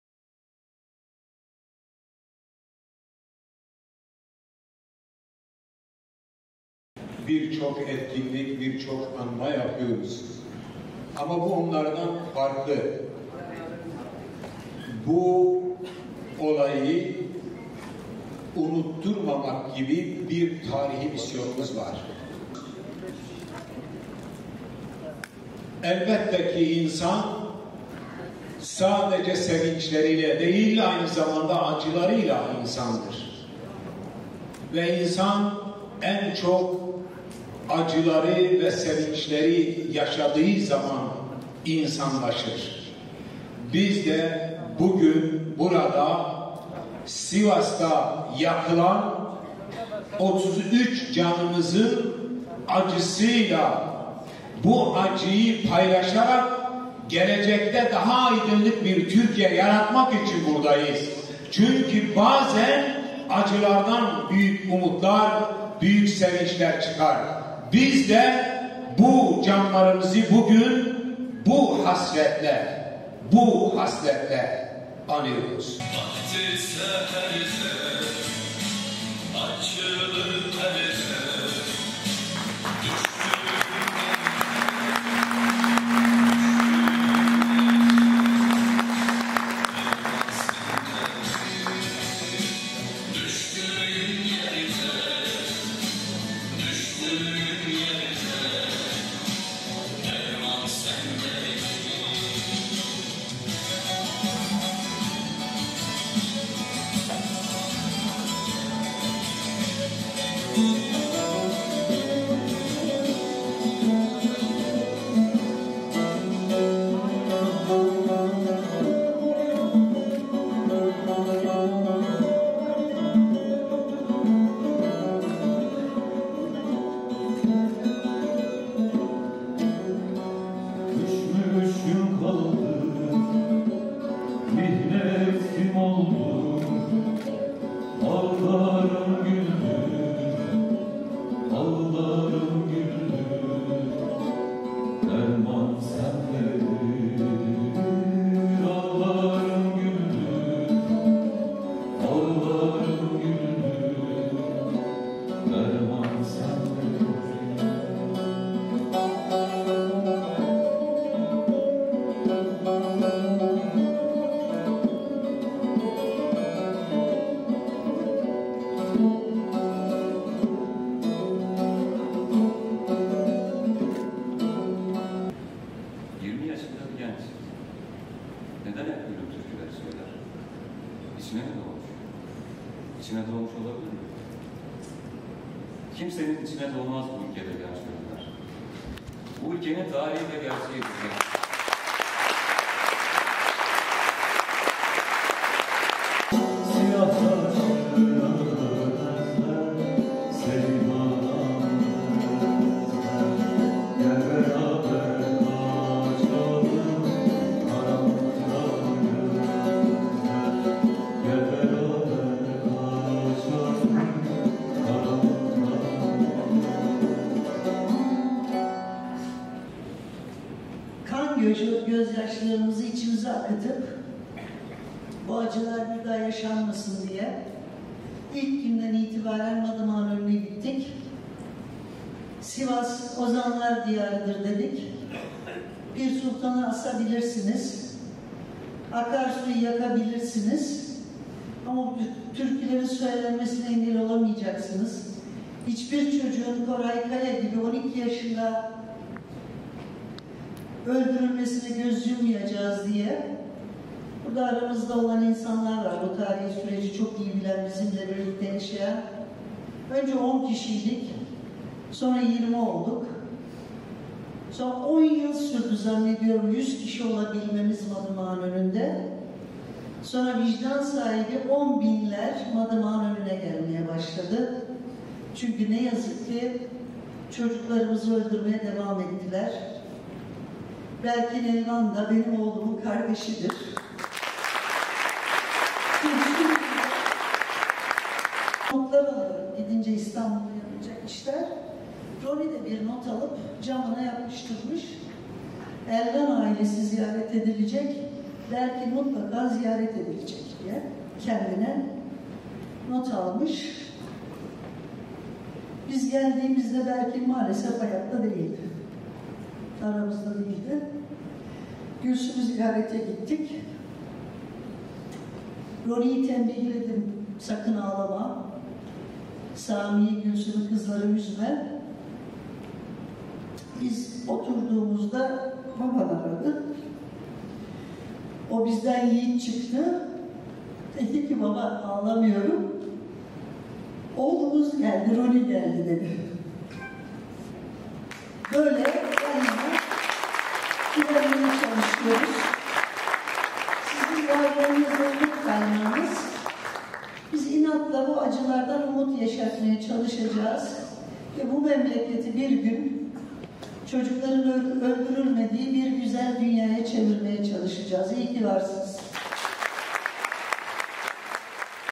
Esenyurt’ta ‘Hasret Gültekin Türkü Müzikali’ gösterimi yapıldı
PİRHA - Esenyurt Nazım Hikmet Kültür Merkezi'nde Sivas Katliamı'nda yaşamını yitirenler için anma yapıldı. Hasret Gültekin’in eşi Yeter Gültekin, yaptığı konuşmada “Pir Sultan’ı asabilirsiniz, Akarsu'yu yakabilirsiniz ama türkülerin söylenmesine engel olamayacaksınız. Önce 10 kişiydik sonra 20 olduk. Madımak’ın önünde 100 kişi olabilmemiz 10 yıl sürdü. Ve sonra vicdan sahibi on binler Madımak’ın önüne gelmeye başladı” ifadelerini kullandı.